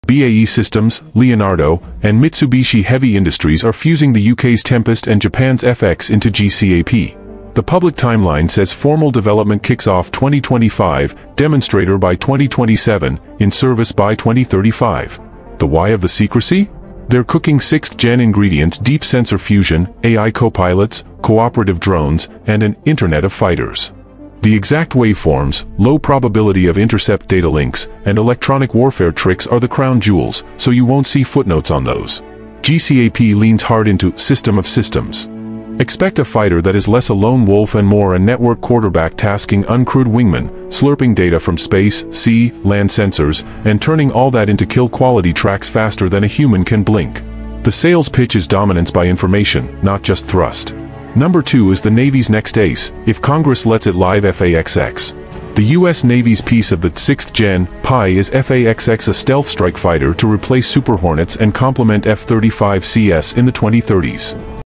GCAP & Tempest The Avengers of Aerospace Are
Get ready for the ultimate aerospace Avengers! This clip reveals how UK, Japan, and Italy are teaming up to create GCAP, a sixth-gen fighter network with AI copilots and drone wingmen. Perfect for tech lovers and future warfare enthusiasts eager to see how teamwork in the sky will dominate battles. Don’t miss this futuristic squad reveal—share the future now!